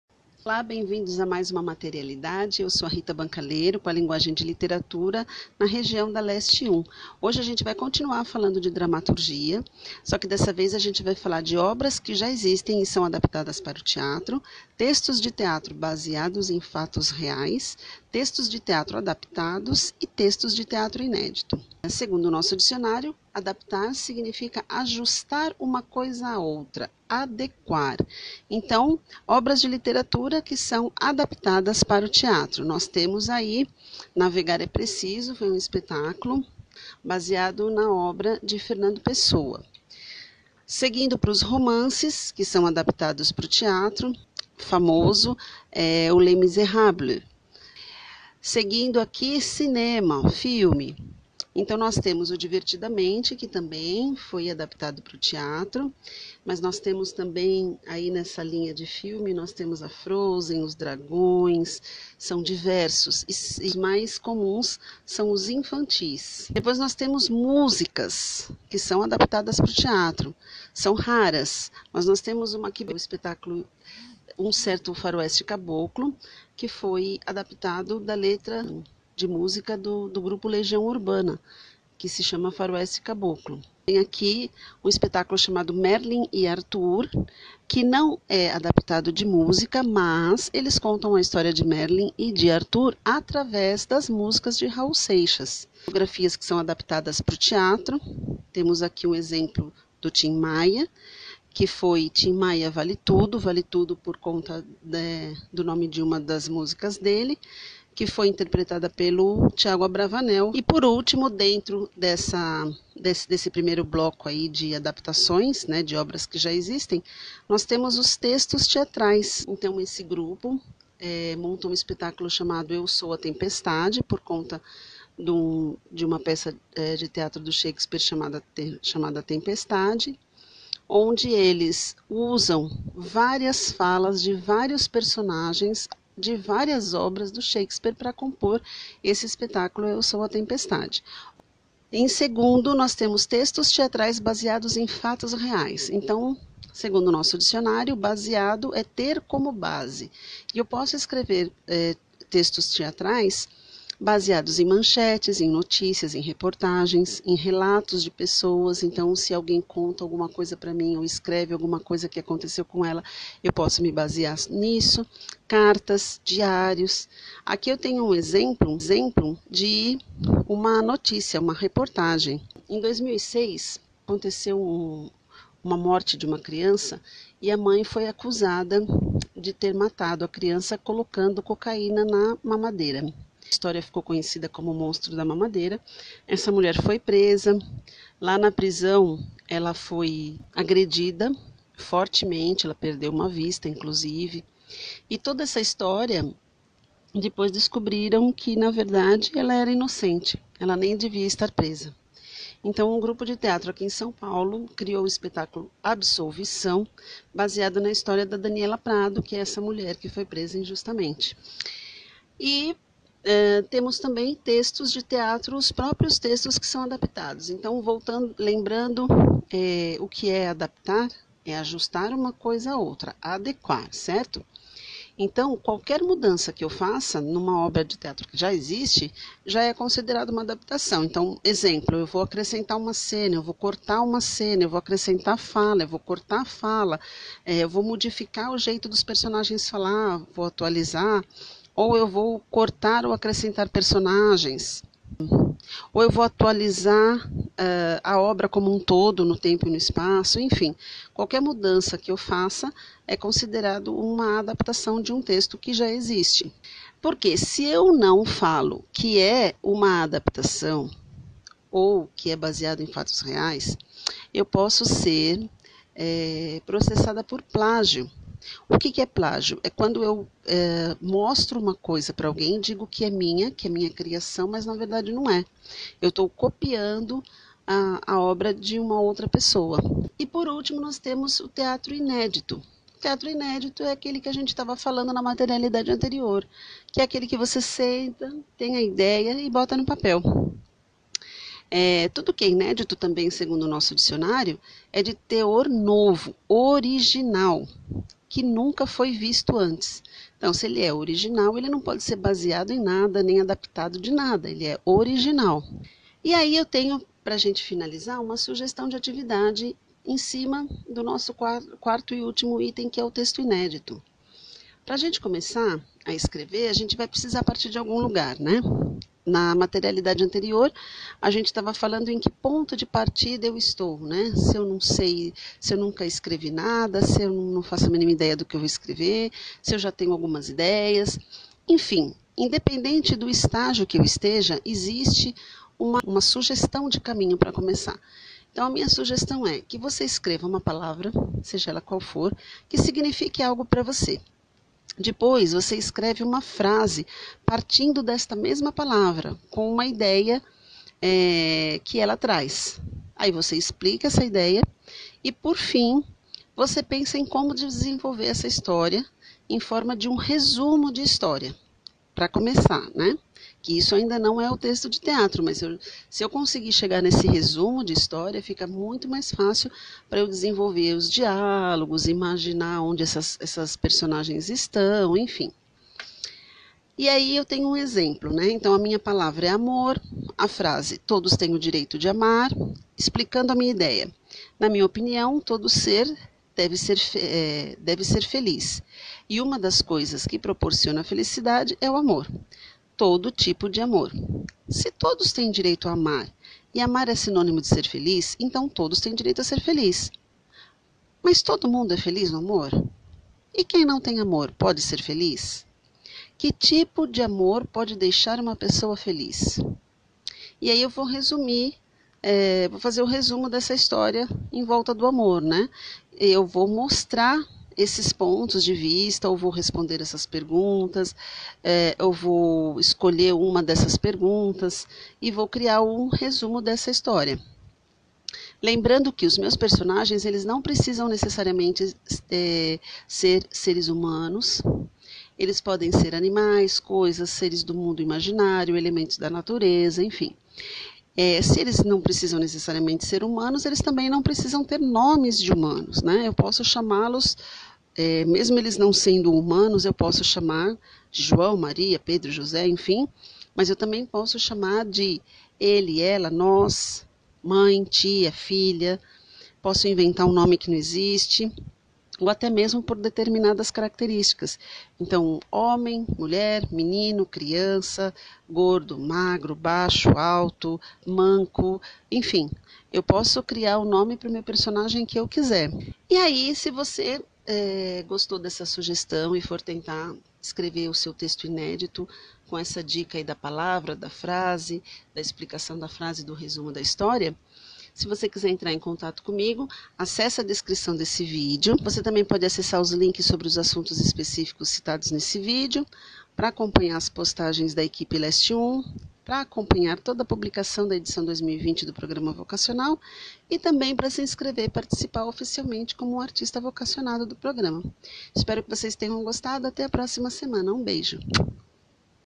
DRAMATURGIANDO
Neste 19º encontro, a artista orientadora, Rita Bancalero (em continuidade das materialidades  17 e 18), fala sobre ADAPTAÇÕES, TEXTOS INÉDITOS e TEXTOS BASEADOS EM FATOS REAIS, com exemplos e sugestões de caminhos para a escrita de uma dramaturgia inédita.